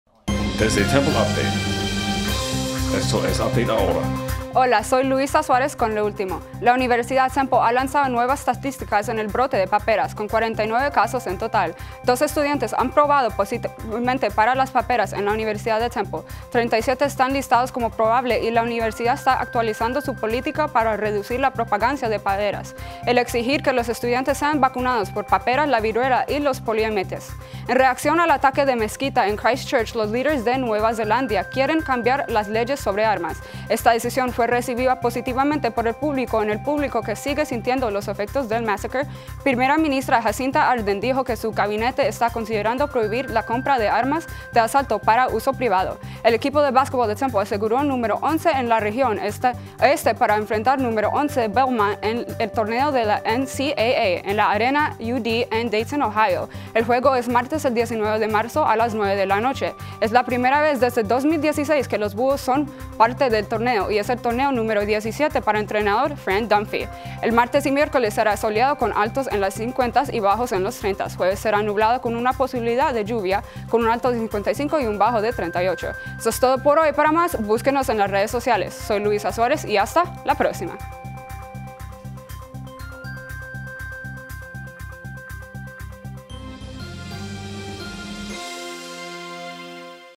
Update Ahora 18 Marzo 2019
Desde el Cuarto de Noticias de Temple Update, esto es Update Ahora.
Aquí están las ultimas noticias:
- 49 casos de paperas
- Nuevas reglas de armas en Nueva Zelanda
- La Locura de Marzo en Temple